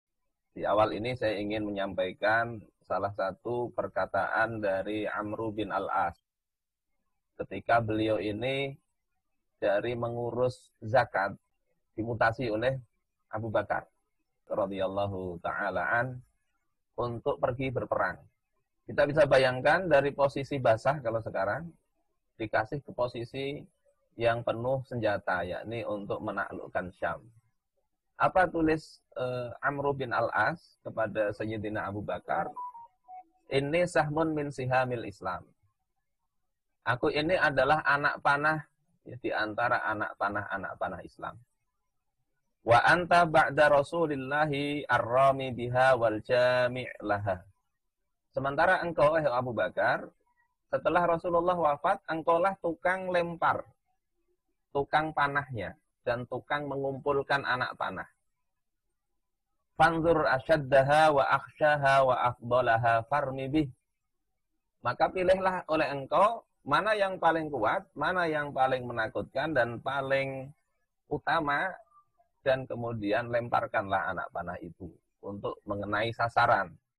Sikap Sahabat Saat Dimutasi dari Tempat Basah ke Medan Perang
Bukan sembarang mutasi, ini mutasi dari jabatan "basah", ngurus duit, dipindah ke medan jihad... beginilah sikap sahabat Amr bin al-Ash r.a